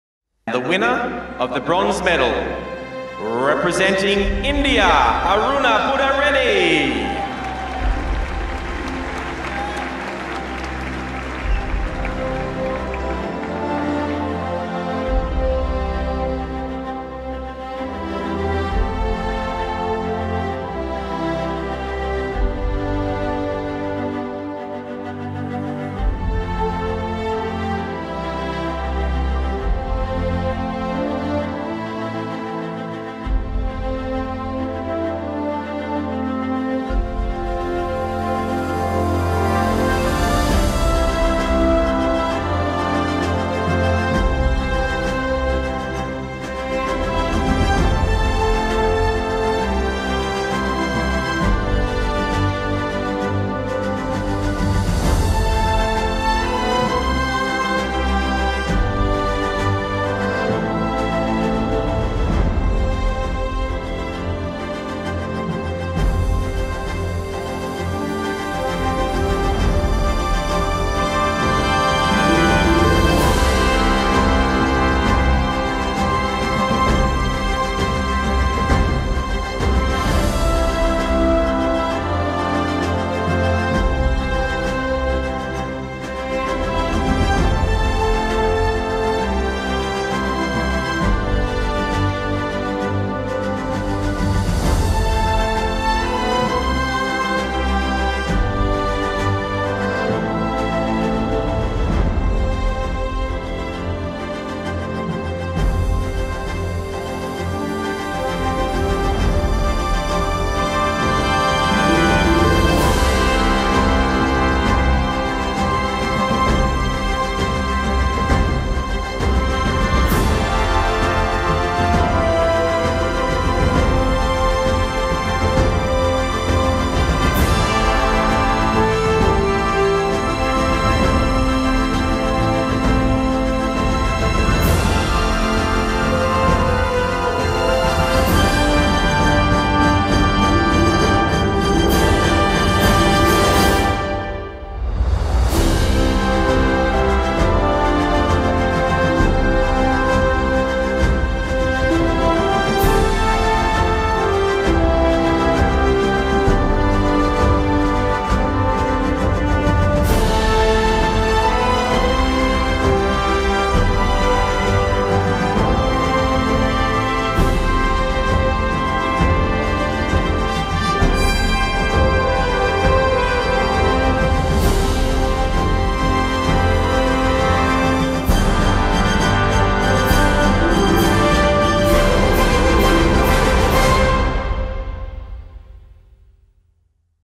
Indiatimes - Aruna Reddy Won The Bronze At The Gymnastics World Cup
Indian Gymnast Aruna Reddy recently bagged the Bronze Medal in the Women's Vault event at the Gymnastics World Cup held in Melbourne. Another Indian woman making us so proud yet again. More power to her!